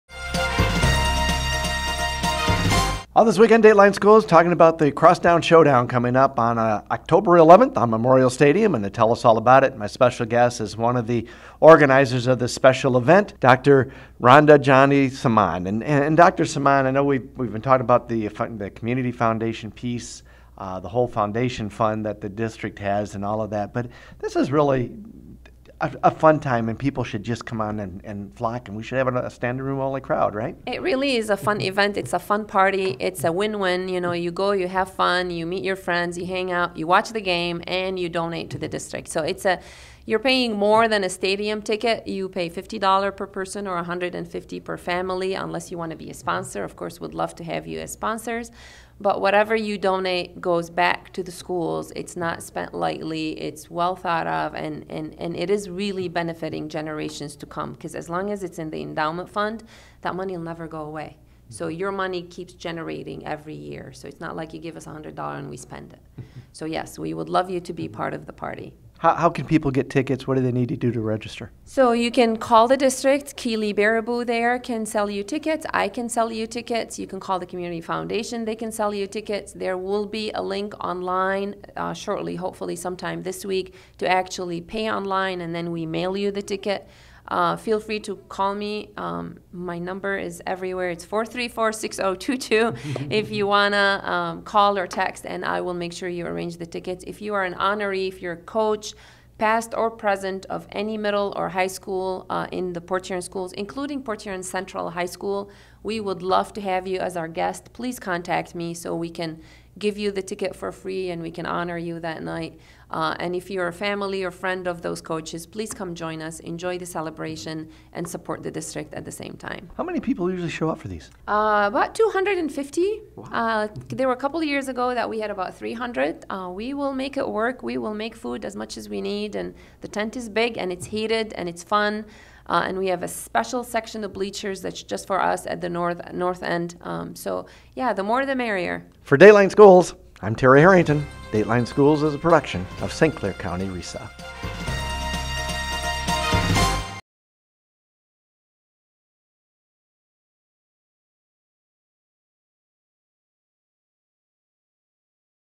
Dateline Schools Crosstown Showdown Friday
This week on Dateline Schools we talk to Dr. Randa Jundi Samman about the Crosstown Showdown and how the money raised will help the school district in the future.